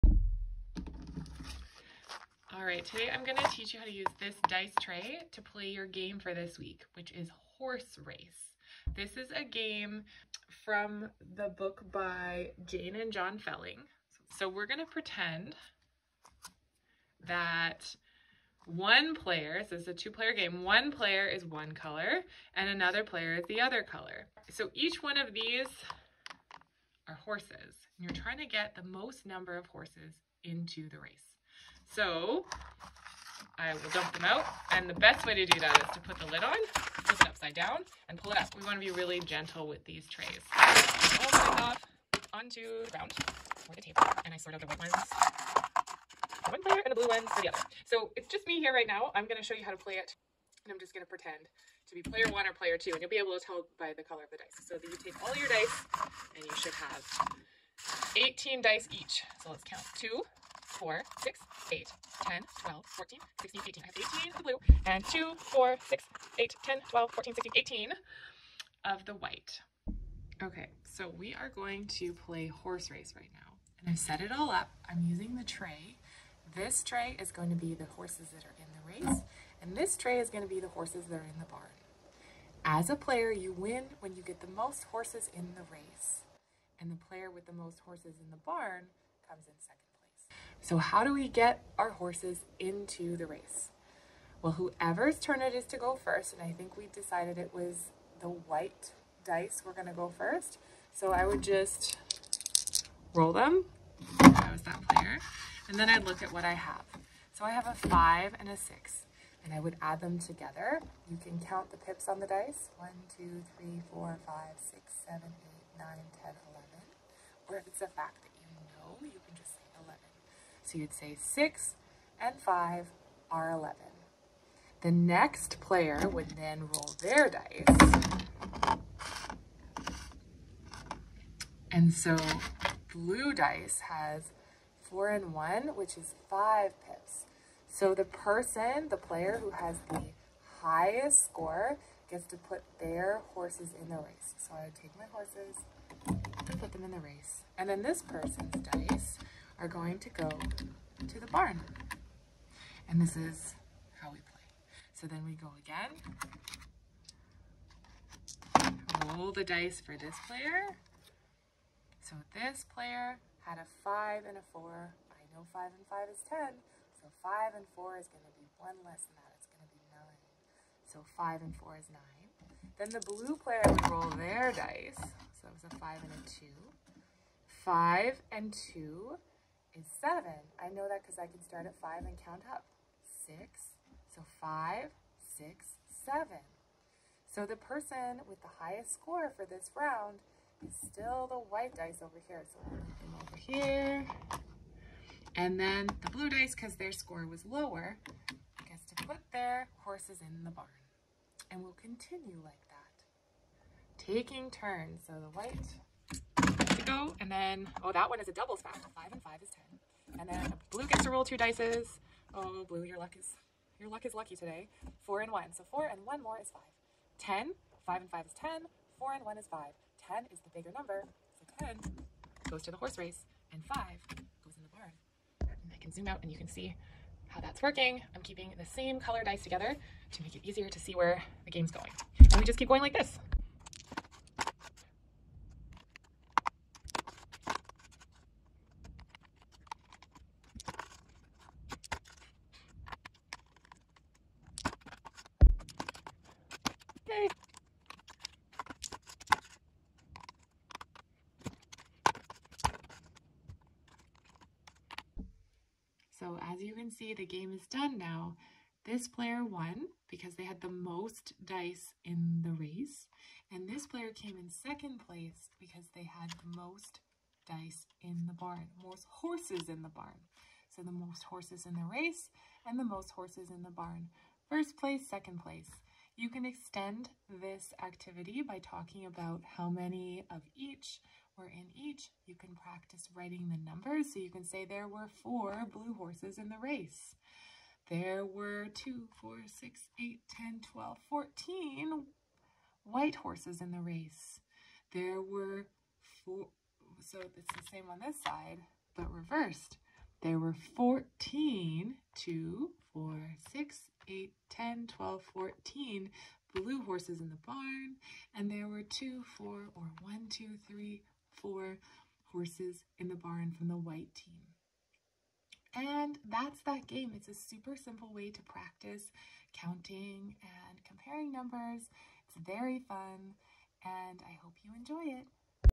horse race
This video explains how to play a game called horse race with your dice tray.

It is a good way to practice mental math facts  to 12 as well as comparing (ordering)  numbers.

For extra learning encourage players to say their equations out loud. "I have 5 and 6, 5 plus 6 makes 11"

Jane Felling is a gifted teacher, you can find her work at the has so many fantastic resources on it.